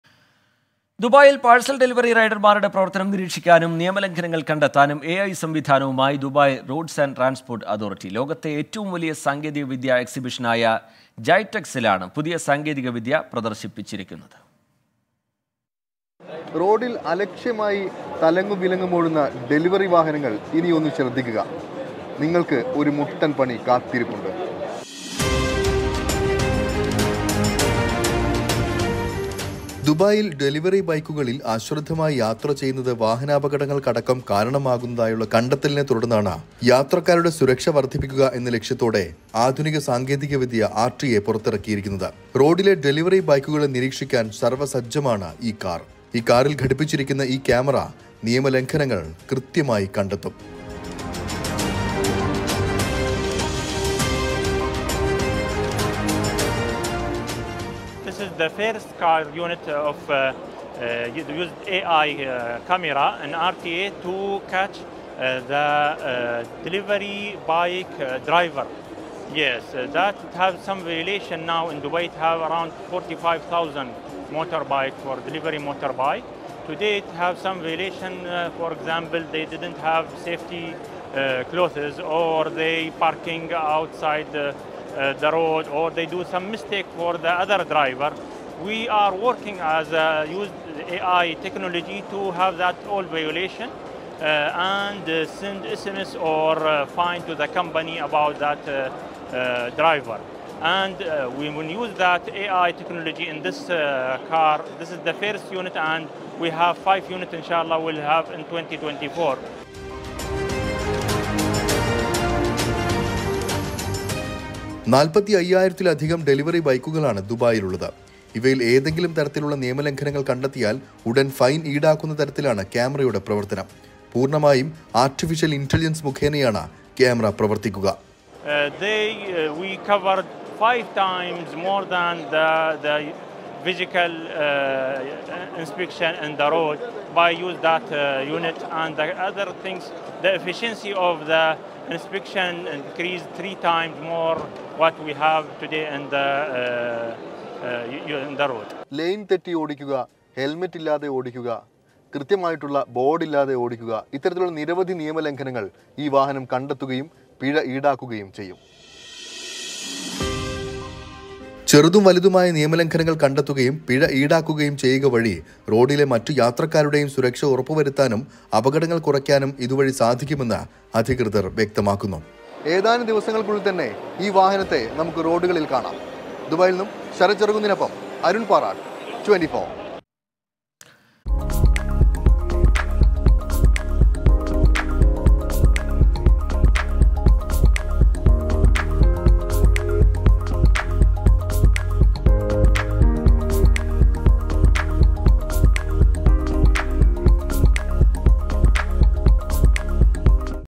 ദുബായിലെ ഡെലിവെറി വാഹനങ്ങൾ സൂക്ഷിക്കുക; മുട്ടൻ പണി വരുന്നുണ്ട്‌
ദുബായിൽ പാഴ്‌സൽ ഡെലിവെറി റൈഡർമാരുടെ പ്രവർത്തനം നിരീക്ഷിക്കാനും നിയമലംഘനങ്ങൾ കണ്ടെത്താനും AI സംവിധാനം

dubai, ai